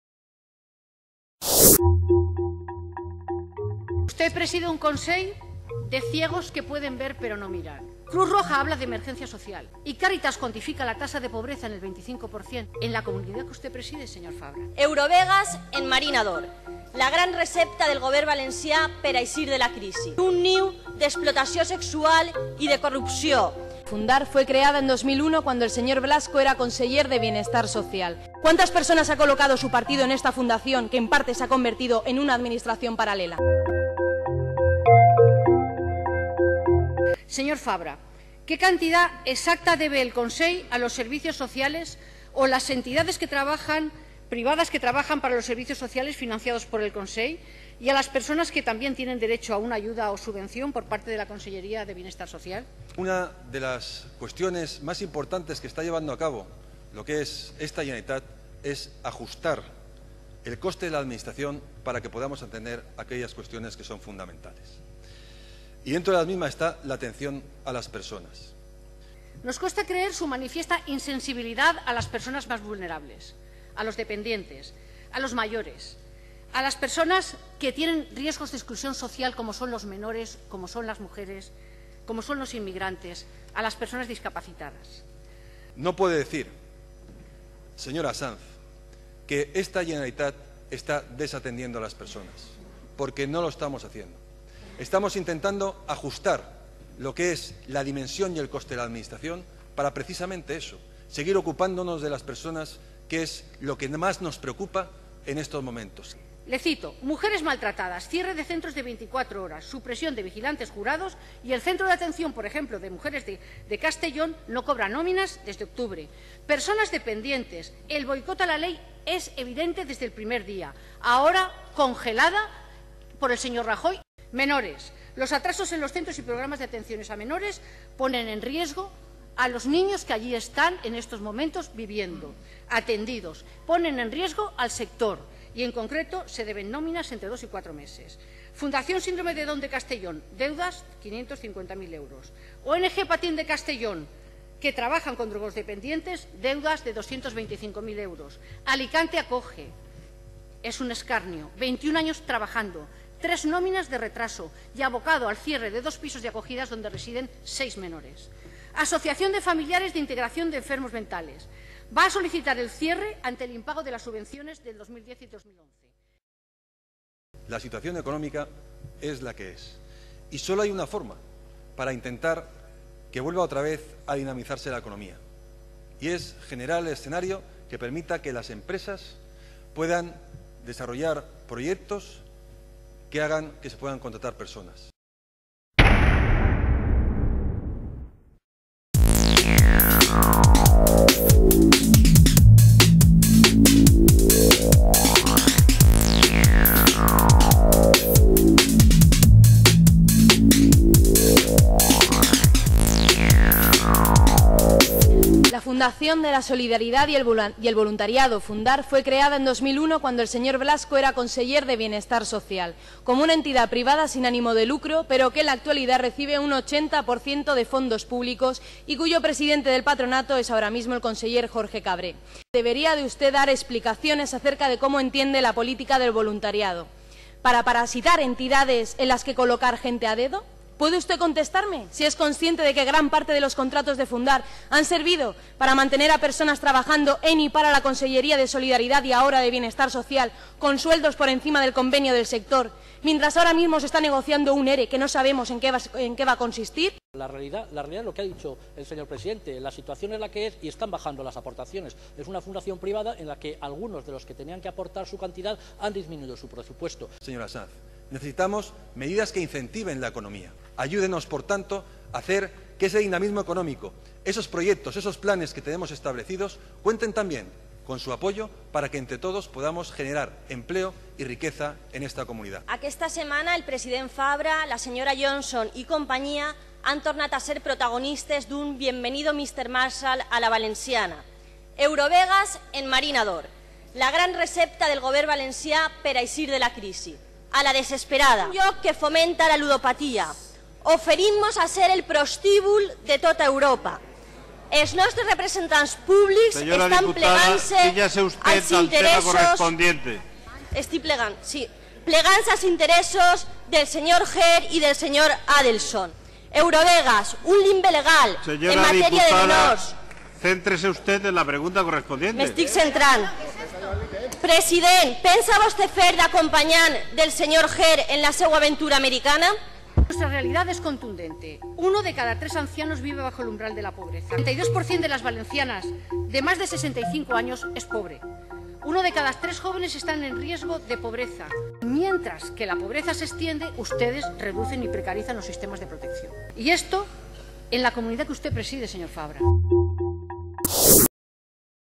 Esquerra Unida pregunta pel deute de la Generalitat amb entitats que presten serveis socials
Els continus impagaments de la Generalitat Valenciana als serveis socials, que s'arrosseguen des de fa anys i que el Consell ha agreujat amb les retallades, estan portant a nombrosos col•lectius a una situació d'asfíxia crònica i insuportable que afecta tant a les persones i als grups socials més vulnerables de la societat -dependents, malalts mentals, víctimes de maltractaments, etcètera-, com a les persones que treballen en el sector de l'assistència social i de les cures.

La síndica del Grup Parlamentari Esquerra Unida pregunta al president de la Generalitat, Alberto Farba, pel deute públic amb les entitats que presten aquests serveis socials (Ple de les Corts del 7 de juny de 2012).